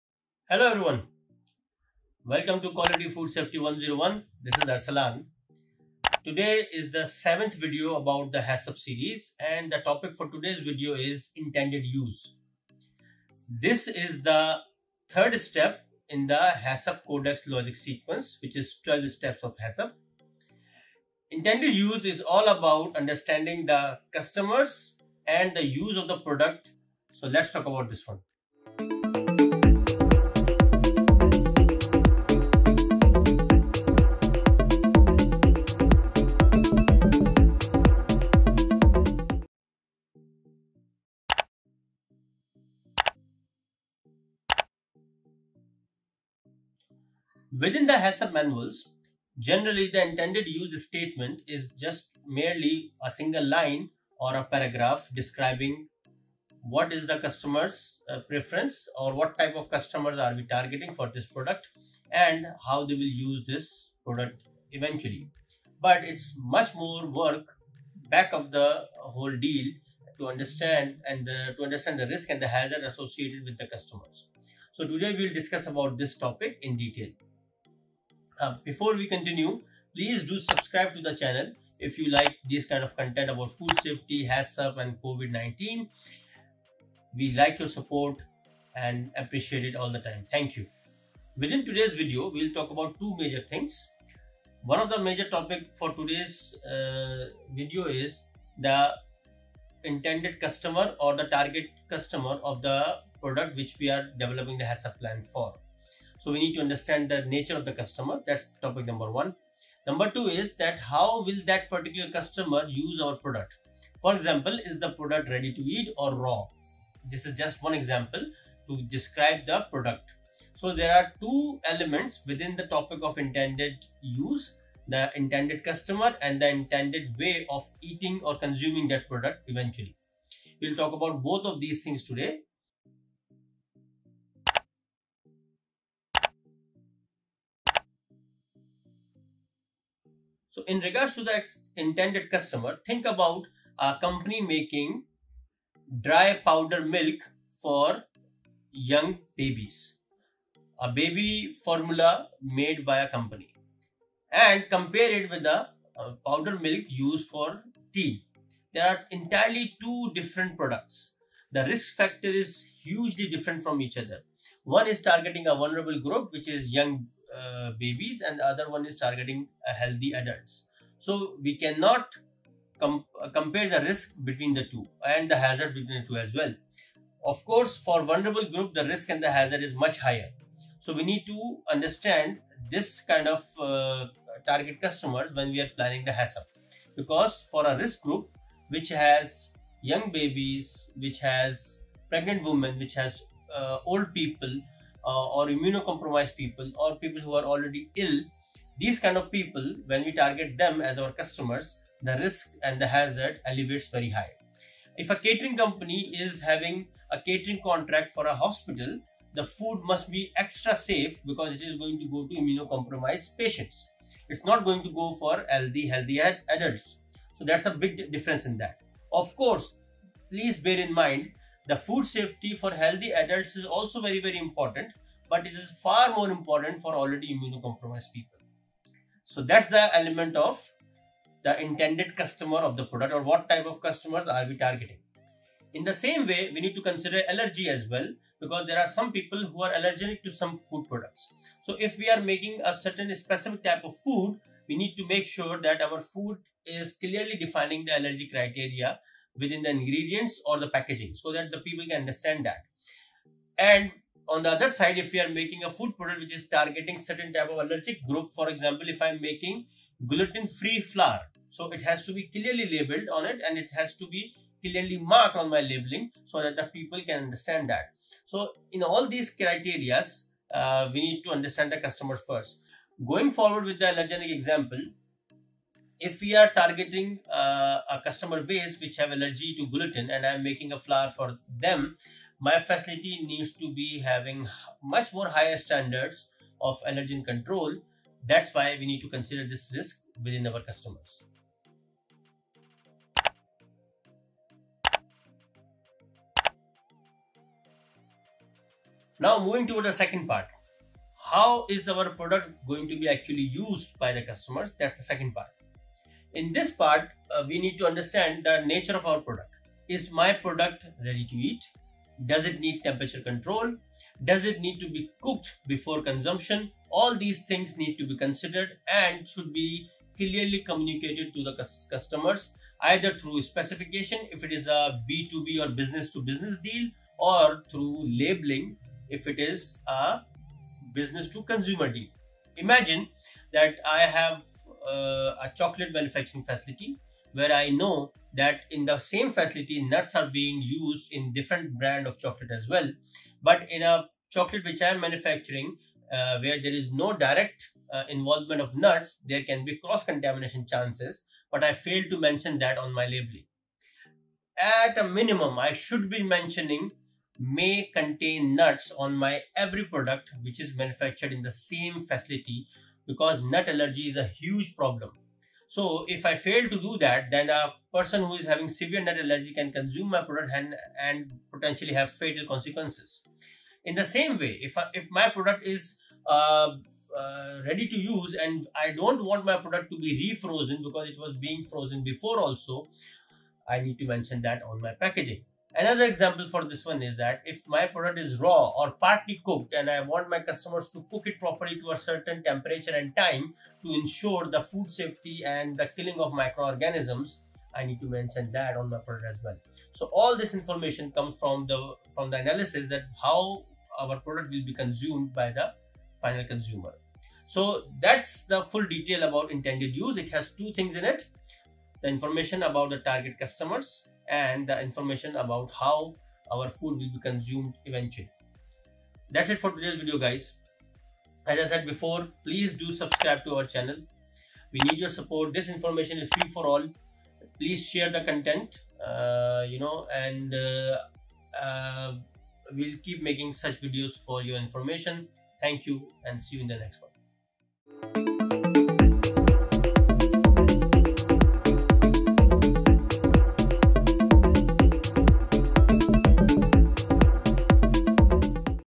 HACCP episode 7 - INTENDED USE (Step 3 in HACCP)
HACCP is Hazard Analysis Critical Control Point. This is episode 7 of HACCP videos which covers details about INTENDED USE.
Product and Process Description is the second step in Codex Logic Sequence (12 Steps of HACCP). This series of videos will cover complete details of HACCP.

00:00 Intro
00:40 Intended Use
02:14 Understanding the Customer
04:52 Use of Product (by the Customer)
06:47 Outro